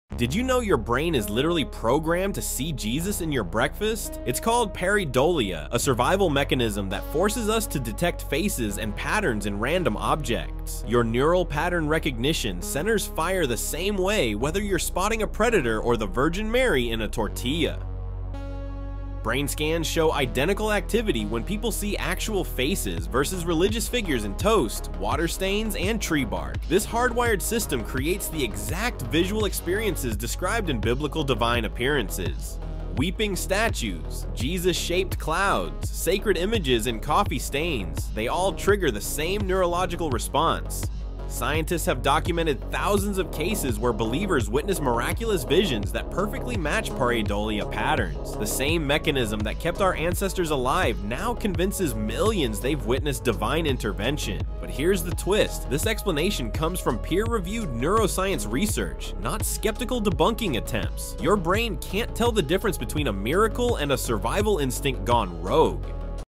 Your Brain Is Programmed to See Jesus in Toast (The Science Will Shock You)
brain sees faces everywhere even jesus